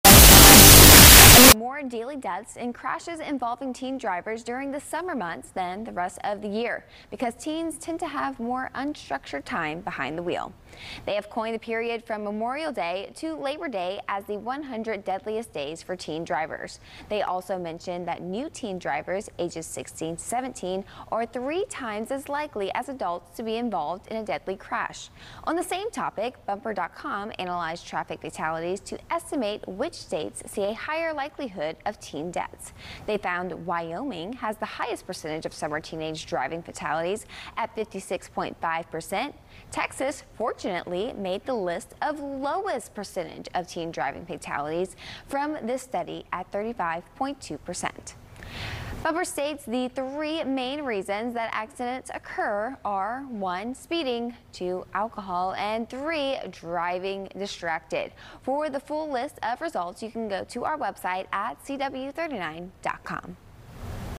Road Rules: 100 Deadliest Days for Teen Drivers
AAA Texas highlights the period from Memorial Day to Labor Day as the "100 Deadliest Days for Teen Drivers". Another study by AAA found that new teen drivers between 16-17 years old are 3 times as likely as adults to be involved in a fatal crash.